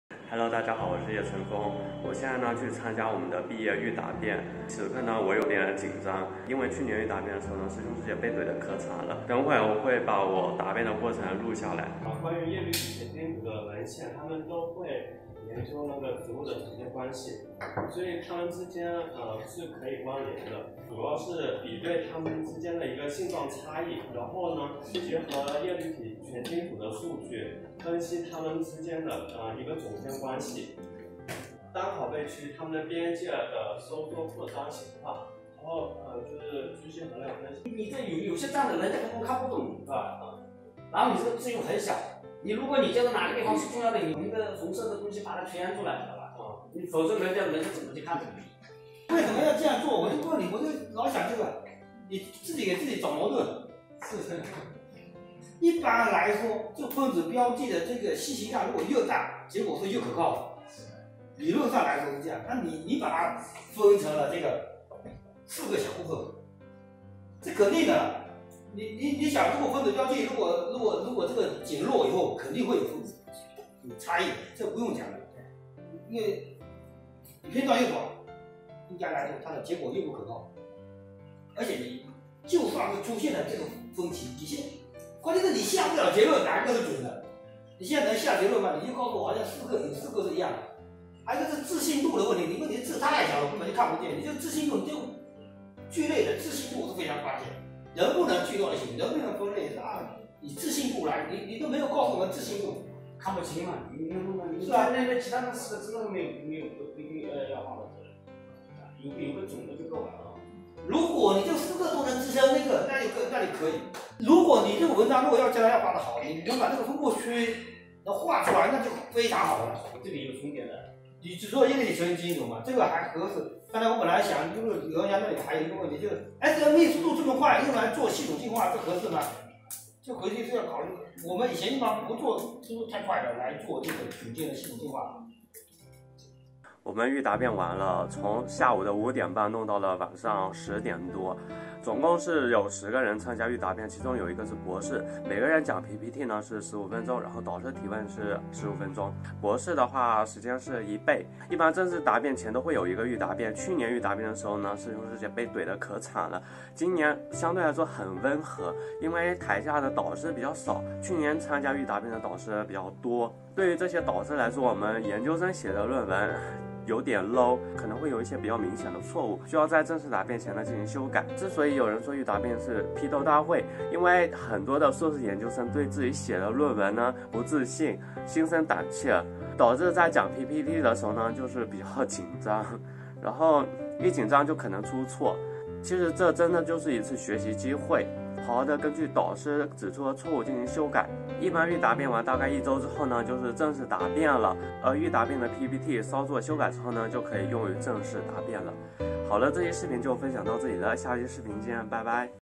研究生毕业答辩前有预答辩，上届师兄师姐被怼的可惨了，现在轮到我了
研究生毕业论文预答辩是什么样的？上一届师兄师姐被怼的可惨了